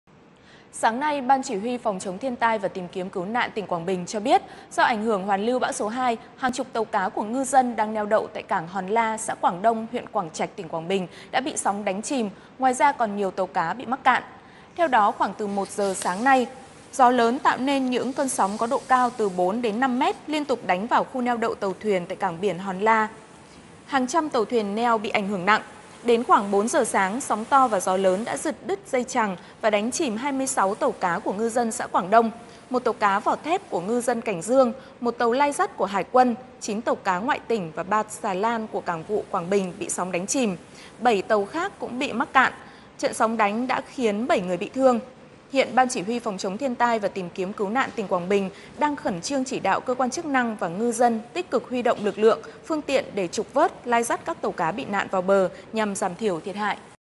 Hàng chục tàu cá của ngư dân Quảng Bình bị sóng đánh chìm
Hàng chục tàu cá của ngư dân Quảng Bình bị sóng đánh chìm Do ảnh hưởng hoàn lưu bão số 2, hàng chục tàu cá của ngư dân đang neo đậu tại cảng Hòn La (Quảng Bình) đã bị sóng đánh chìm.